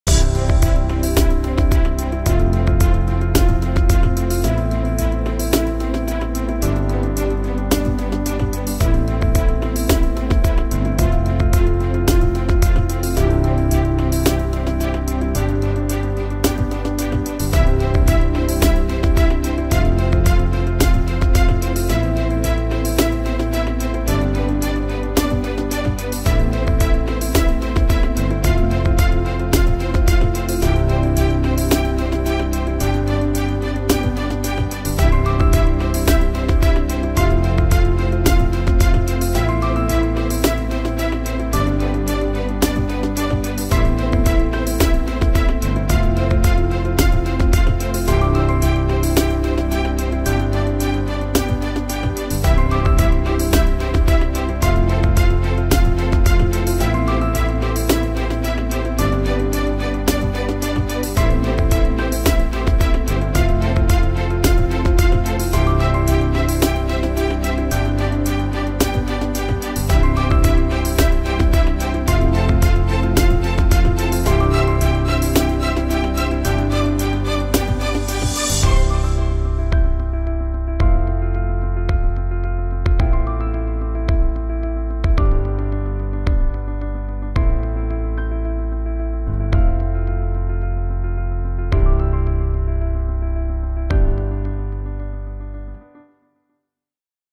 Hövsandakı sosial evlər – BELƏ TƏMİR EDİLİB - VİDEO - FED.AZ
Mənzil İnşaatı Dövlət Agentliyi (MİDA) Hövsan Yaşayış Kompleksindəki sosial mənzillərin təmirini açıqlayıb.
Agentlikdən FED.az-a bildirilib ki, Yasamal Yaşayış Kompleksində olduğu kimi Hövsan Yaşayış Kompleksinin mənzilləri ənənəvi olaraq 2 cür təmir edilib. Təmir növləri dizayn və rəng seçimi ilə fərqlənir. Bütün mənzillər yüksək keyfiyyətli tikinti materiallarından istifadə olunmaqla tikinti standartlarına uyğun zövqlə təmir edilib.